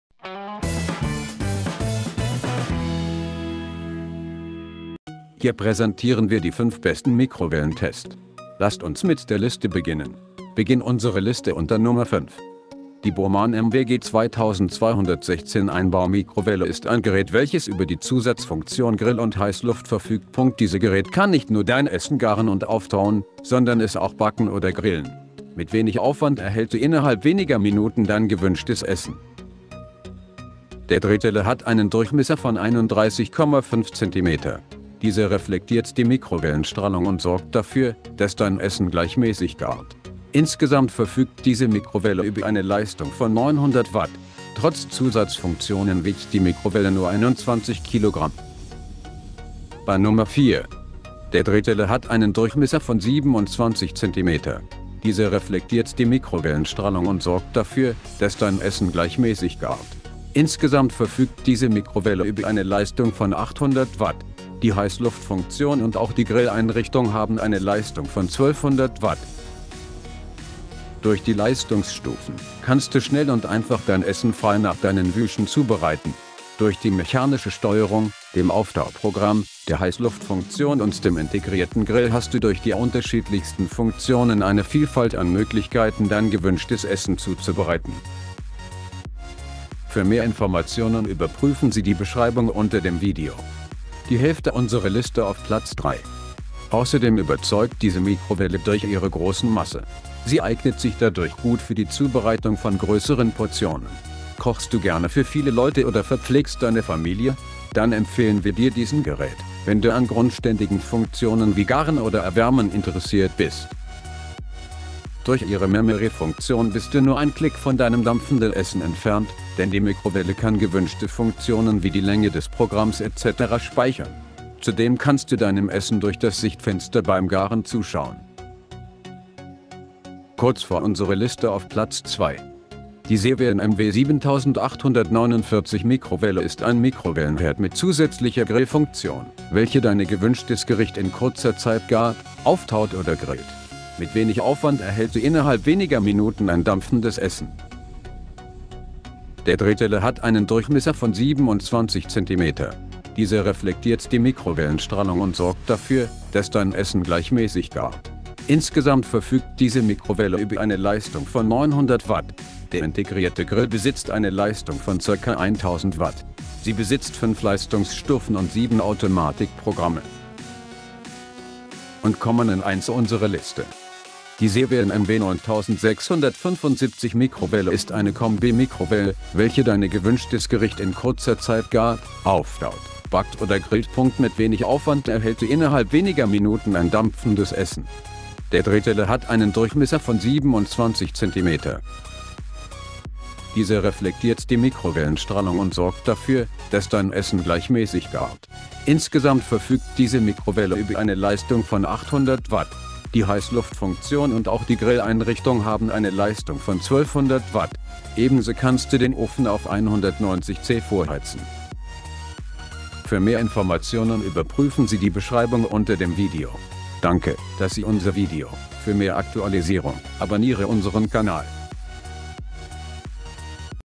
Besten Mikrowellen Test - Updated
Hier ist die Liste von 5  ►  Besten Mikrowellen Test

1. Severin MW 9675 Mikrowelle

2. Severin MW 7849 Mikrowelle

3. Samsung GE-89MST Mikrowelle

4. Clatronic MWG 775 H Mikrowelle

5. Bomann MWG 2216 Einbau-Mikrowelle